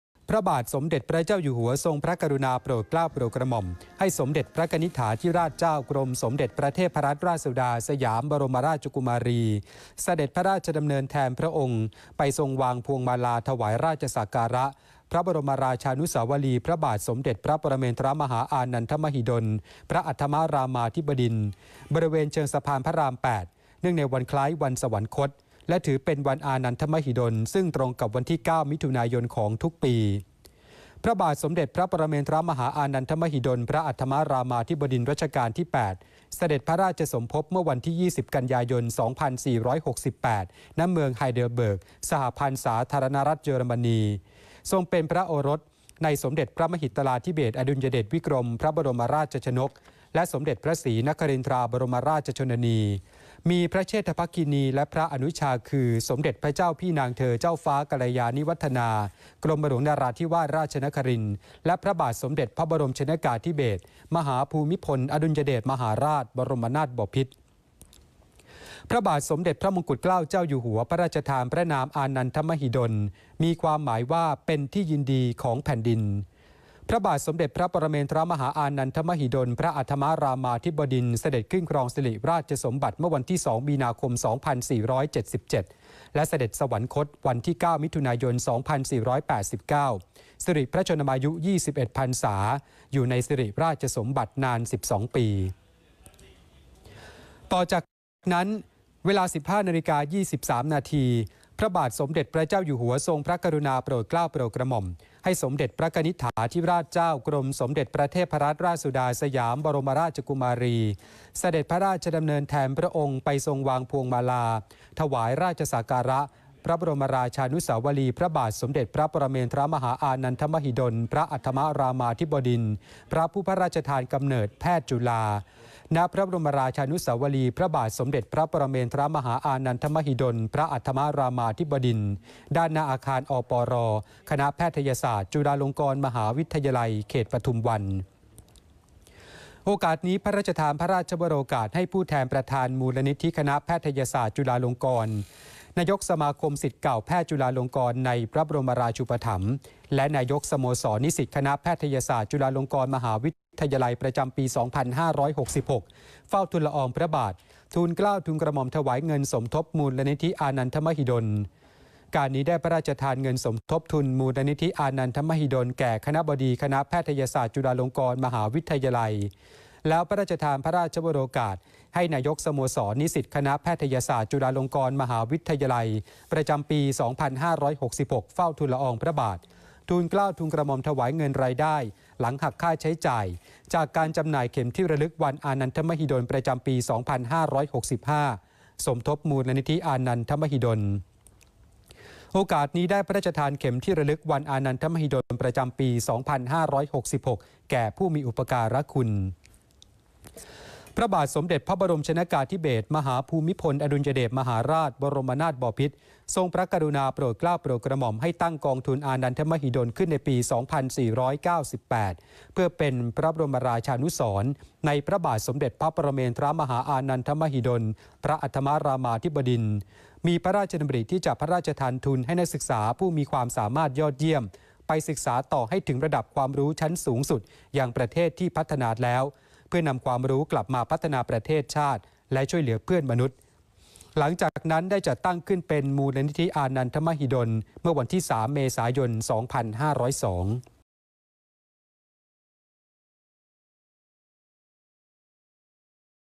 สมเด็จพระเจ้าอยู่หัว โปรดเกล้าสมเด็จพระเทพฯตัวแทน..วางพวงมาลาถวายราชสักการะวัน"อานันทมหิดล ร.8"🙏💜
วันอานันทมหิดล พระเจ้าอยู่หัว สมเด็จพระเทพ พระบาทสมเด็จพระเจ้าอยู่หัวโปรดเกล้าให้ สมเด็จพระกนิษฐาธิราชเจ้า ทรงวางพวงมาลาถวายราชสักการะ ในวันอานันทมหิดล ..พระองค์ทรงพระเจริญ.💜